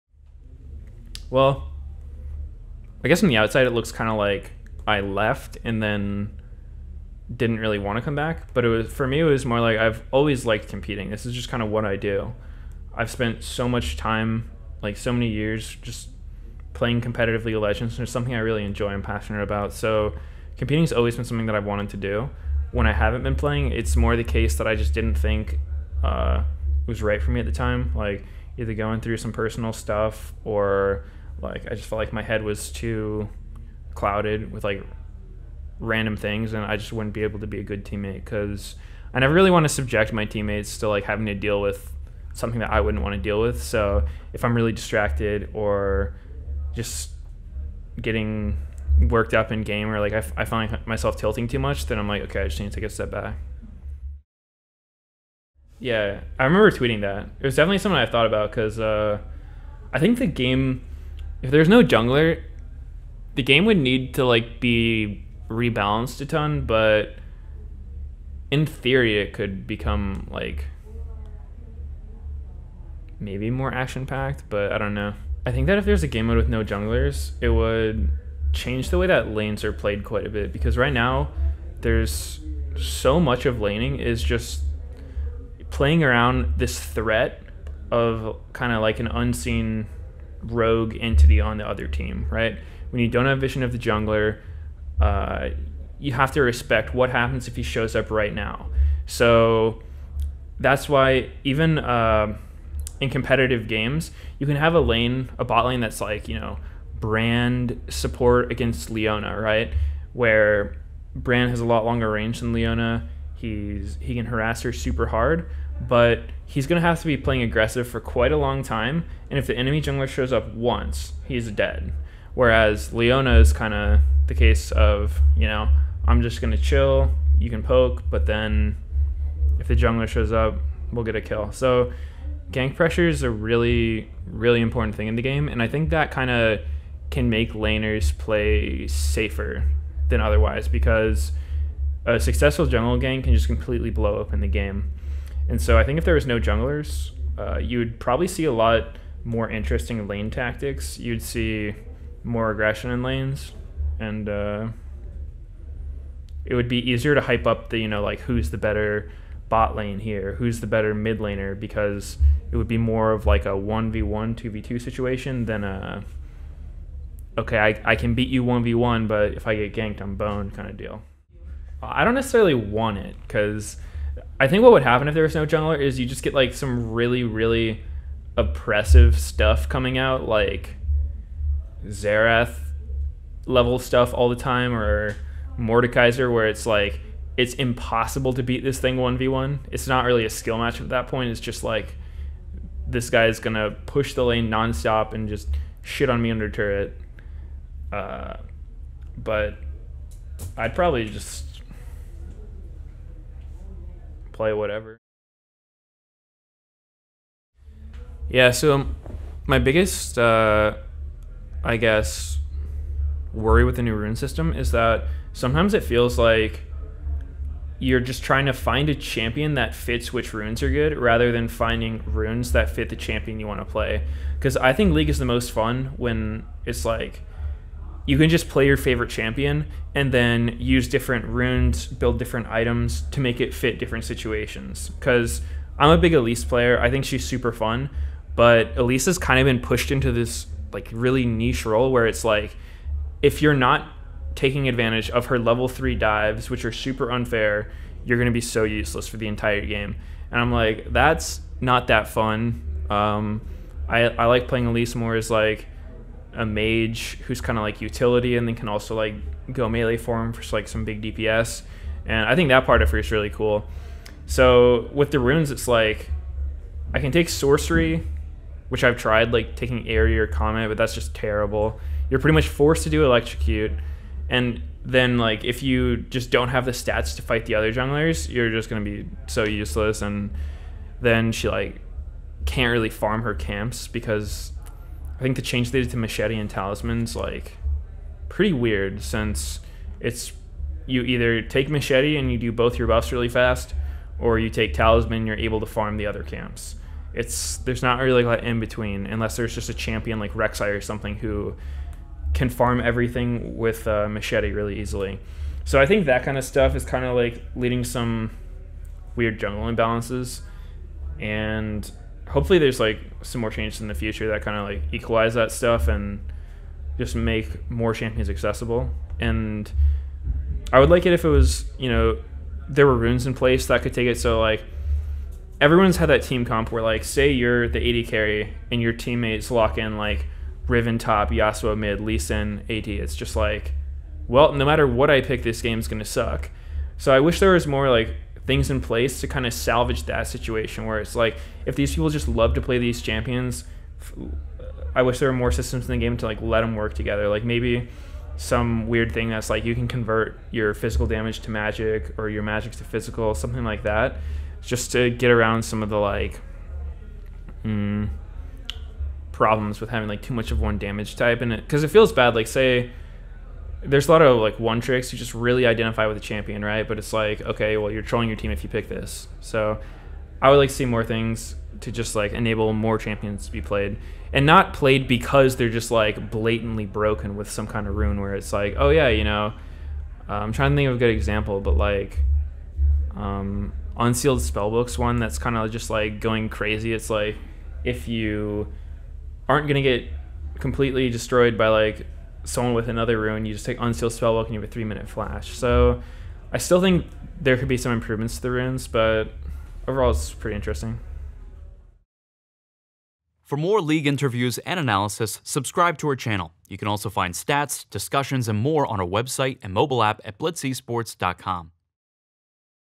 Meteos on how runes could make one-tricks less troll and why he's competing again
Meteos on why he's back to competitive, and problems with the new rune system. Timestamps:

0:00 - Let's talk about why you're back.
0:54 - I'm Riot, pitch me a game mode without junglers.
3:28 - What champions are "invalid" or worse because they don't synergize with the new rune system?